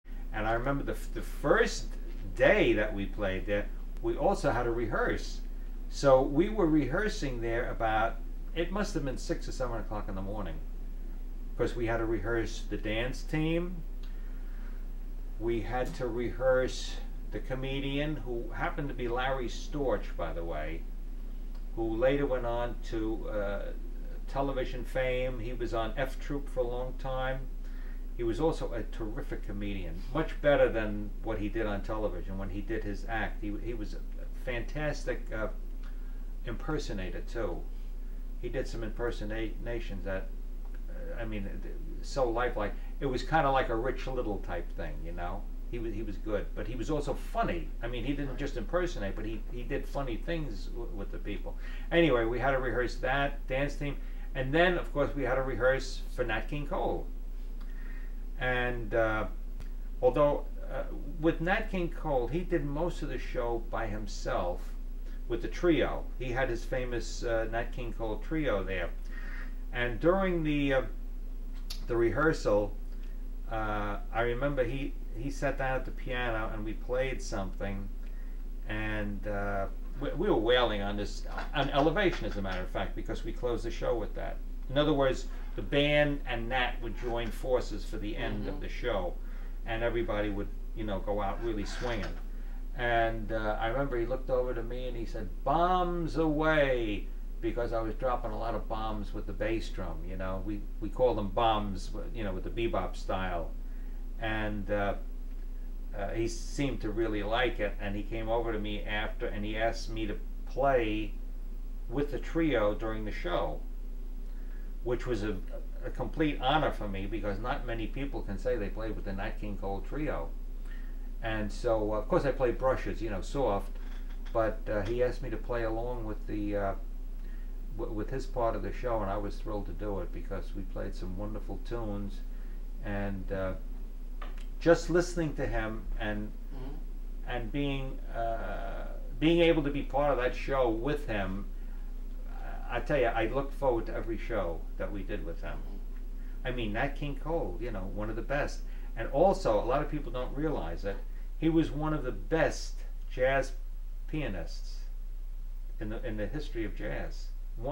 Howie Mann Interview 4
Drummer Howie Mann remembers the Elliot Lawrence Orchestra working three weeks at the Paramount Theater in New York with the Nat King Cole Trio. March 1949.